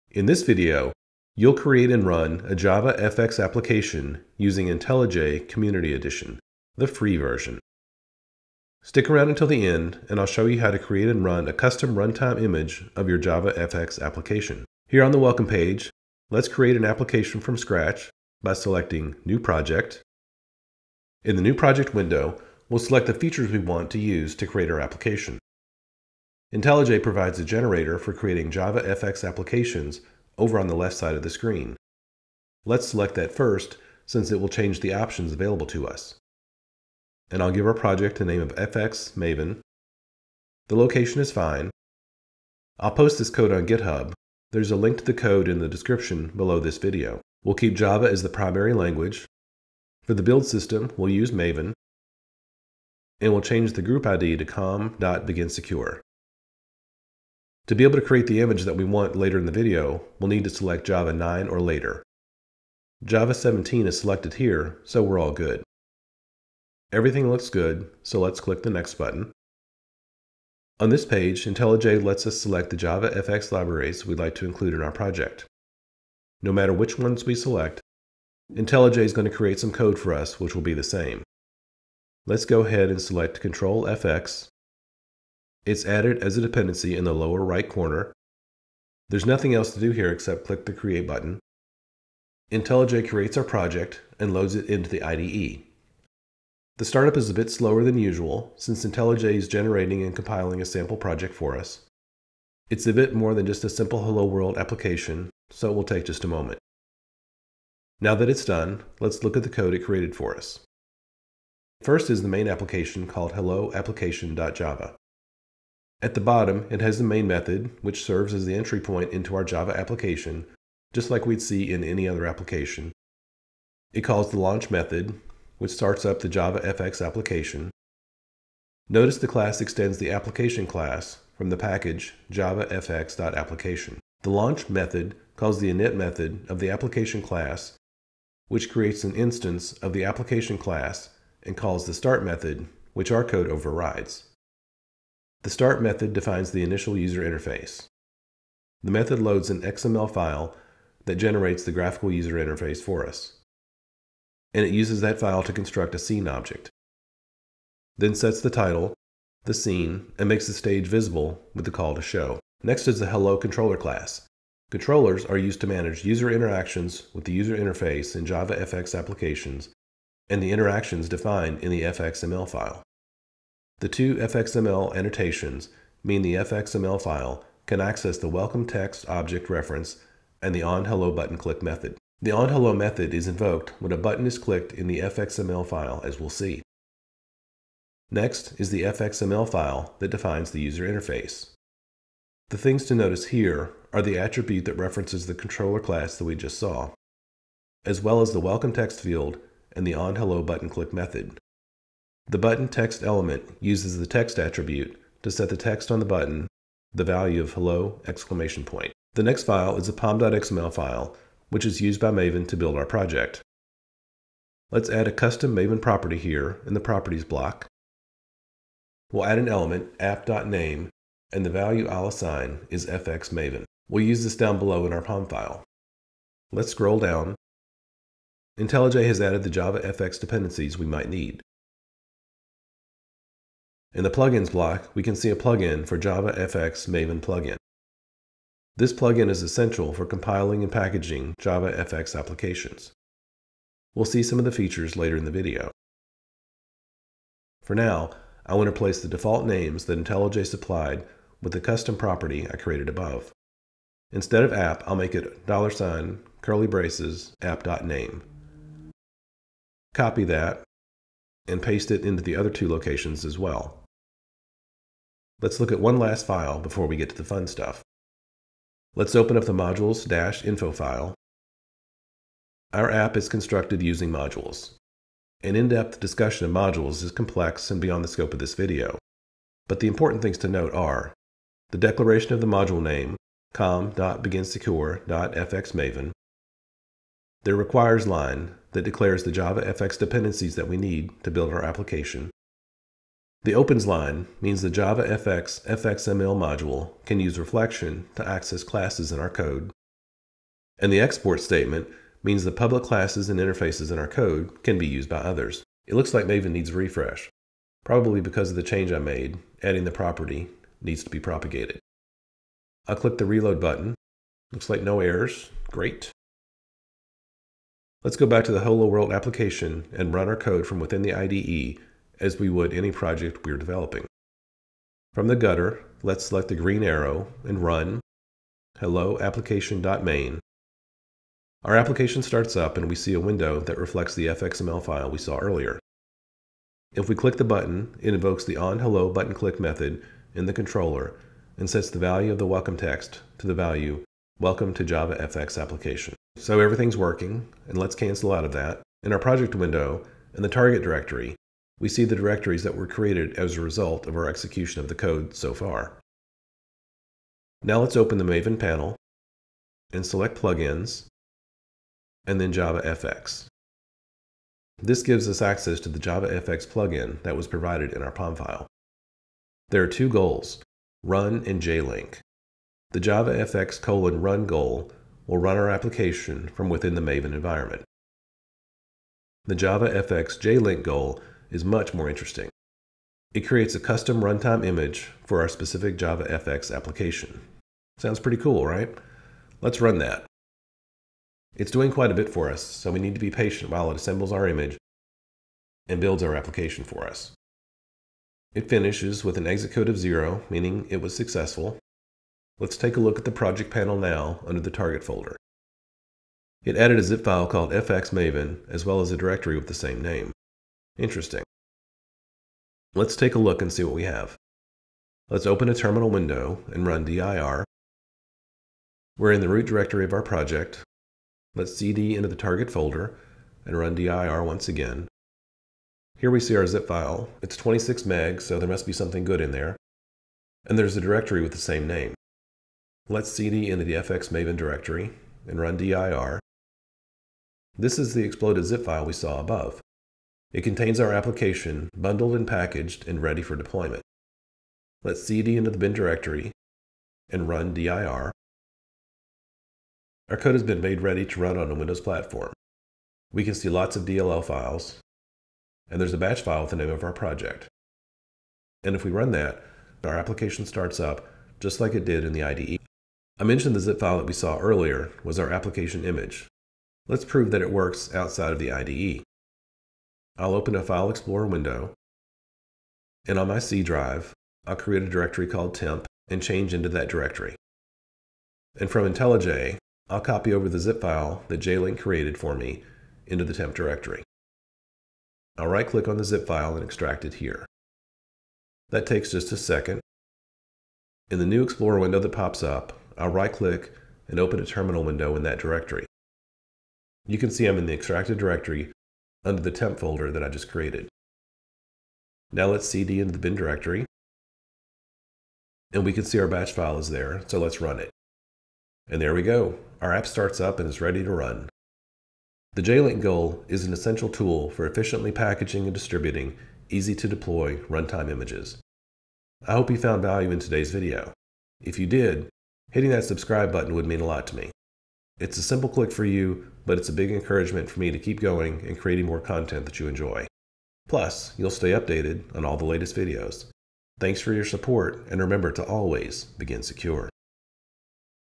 Master JavaFX App Development with Maven and IntelliJ
Join me in this exciting journey as we delve into the world of JavaFX application development using IntelliJ Community Edition. This step-by-step tutorial covers everything from setting up a new JavaFX project named FXMaven to leveraging the power of Maven for building systems. Discover how to create and run a custom runtime image of your JavaFX application, including the use of Java 17 and the jlink tool for optimal performance. We'll explore the intricacies of JavaFX's FXML files and controllers. Learn about the Maven plugins essential for JavaFX applications, the significance of modules in Java, and how to use the javafx:jlink goal for efficient packaging. Watch as we demonstrate running the application in IntelliJ and deploying a packaged application using jlink. Perfect for beginners and seasoned developers alike, this tutorial offers a practical approach to JavaFX development. Don't forget to check out the GitHub link for the code in the description below.

== Chapters ==
00:00 Create JavaFX App with IntelliJ
03:30 Add Custom Maven Property to Pom file
06:05 Build JavaFX image